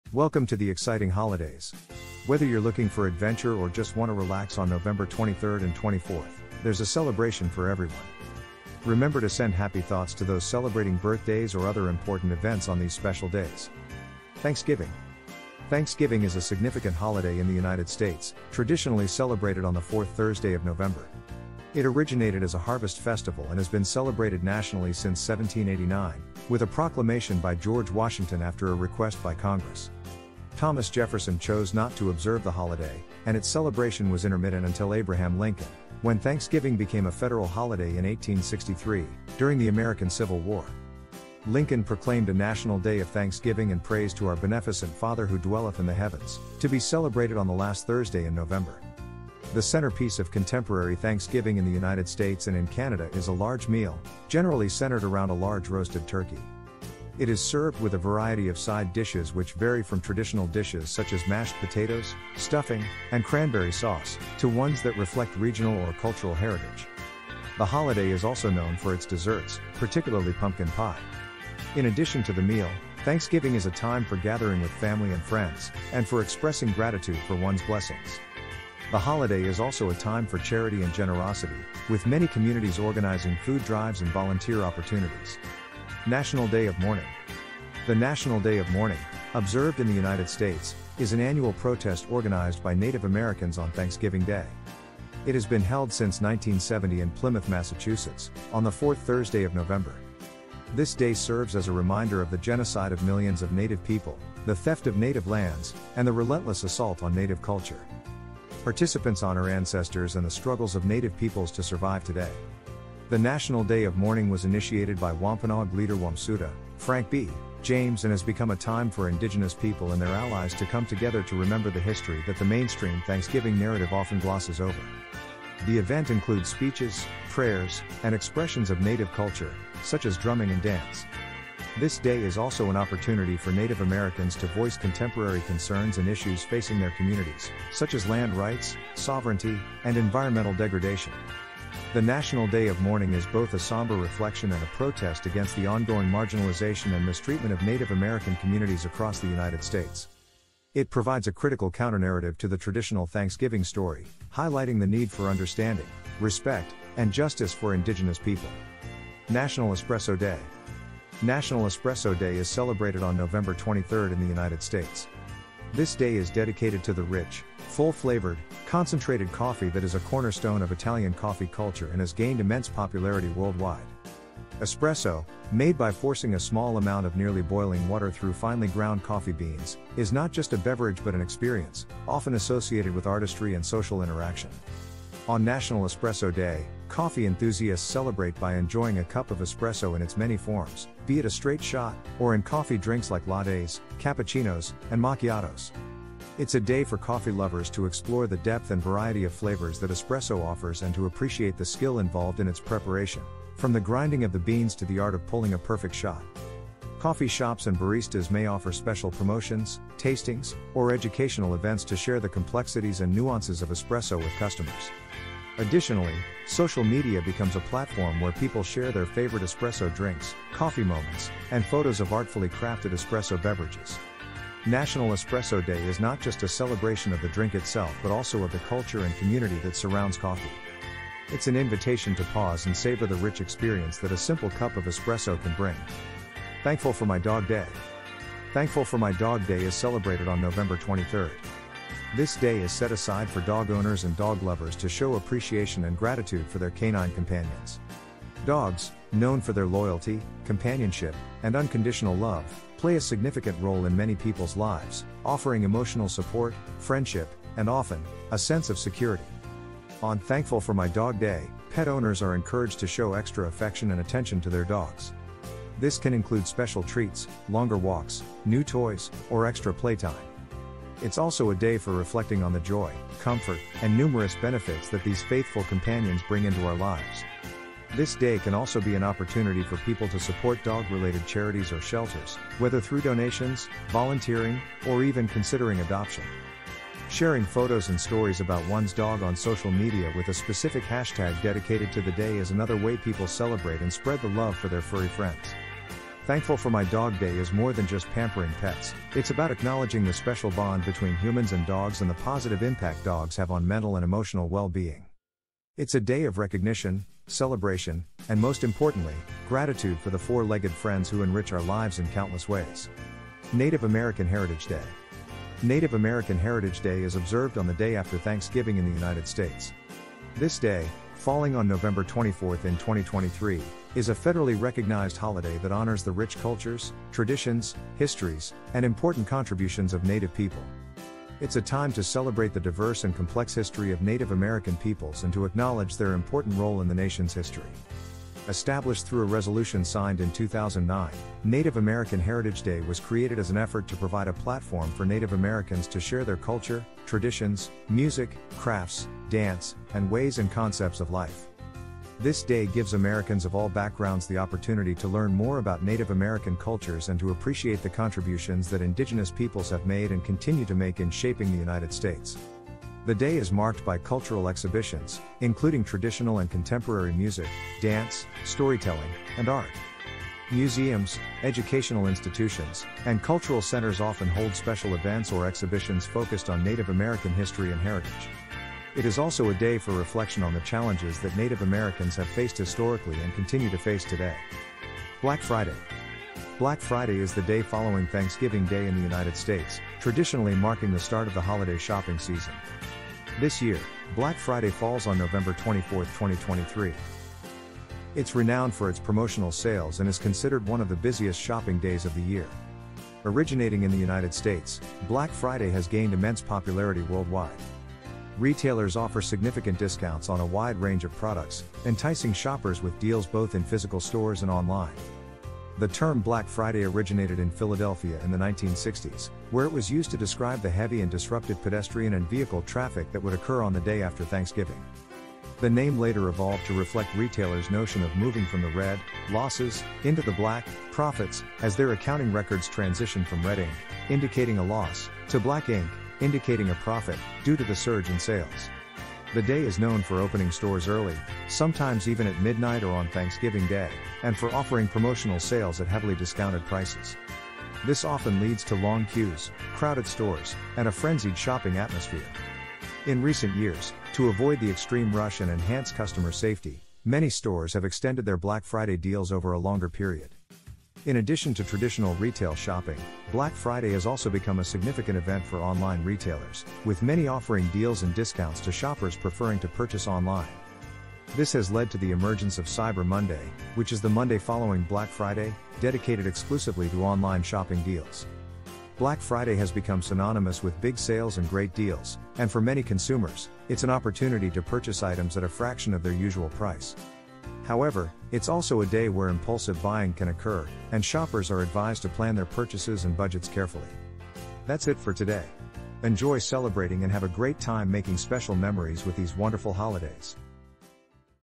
Holidays on Nov 23rd & 24th you can celebrate.
Holidays taking place on Nov 23rd & 24th. Get ready to celebrate with family, friends, or even just take a break for yourself.

Thanksgiving
National day of mourning
National Espresso Day
Thankful For My Dog Day
Native American Heritage Day
Black Friday

This channel is based on facts, rumors & fiction.
The videos have no negative impact on the original works.